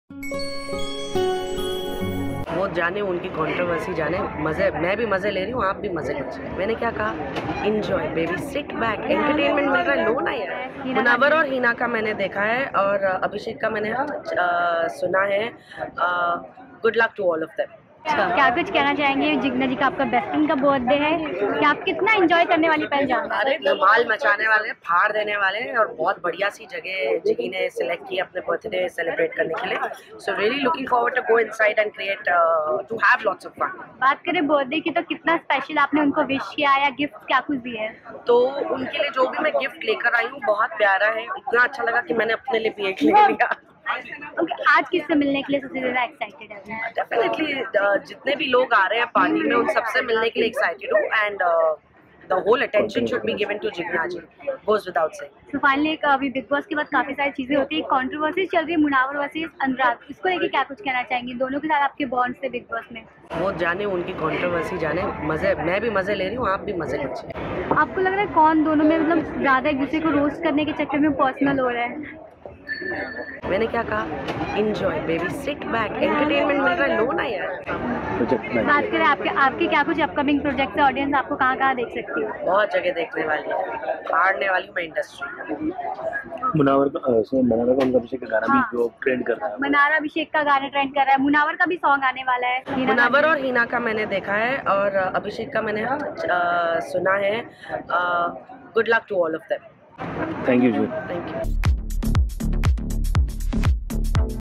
Rinku Dhawa Interview: Munawar Anurag Ke Controversy Par Kiya React, Jigna Ke Liye Laye Gift
Rinku Dhawan Interview: Rinku Dhawan reacts on controversy between Munawar Faruqui and  Anurag Dhobal aka UK07 Rider.